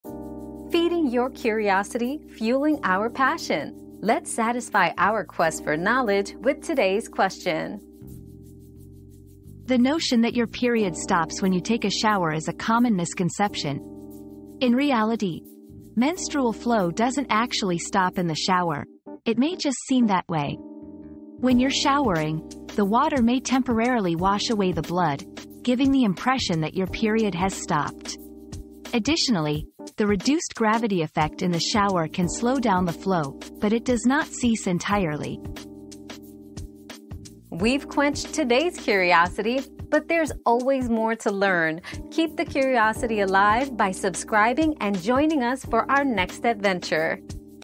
Why does your period stop when you take a shower?
The Shower Myth: Does Your Period Really Stop When You Shower? 👉 The Shower Myth 👉 Discover the truth behind the common misconception that your period stops when you take a shower. Learn why the flow may seem to pause, but actually continues and understand the reduced gravity effect in the shower.

Laura S. Harris (2024, February 6.) Why does your period stop when you take a shower?
    🌐 AskAbout.video